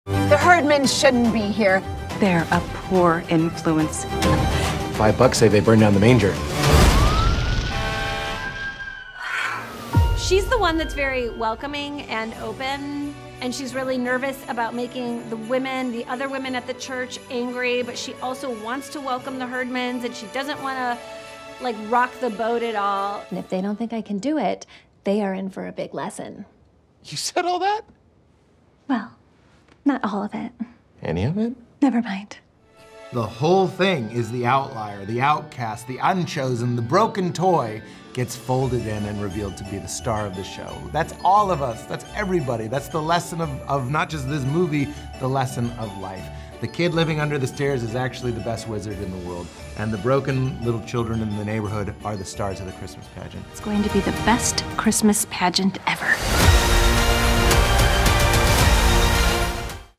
Landmark Exclusive: Best Christmas Pageant Ever
landmarkcinemas chats with Judy Greer and Pete Holmes about The Best Christmas Pageant Ever – Get tickets